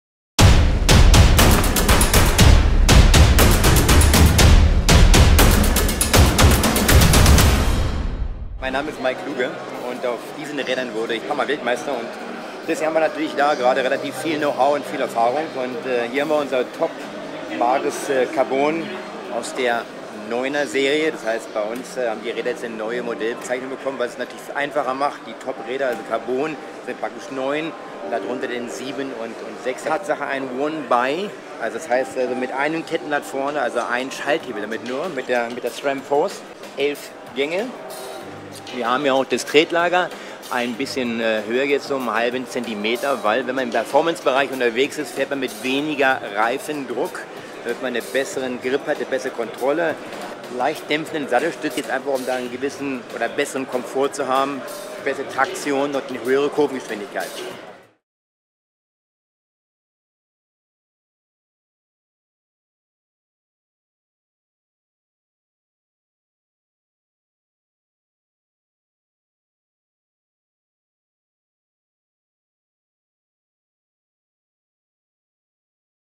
FOCUS Bikes - MARES 2019
FOCUS Bikes founder Mike Kluge presenting the 2019 FOCUS MARES 9.9 Cyclocross bike to you.
Great bike, isn't it? 🔥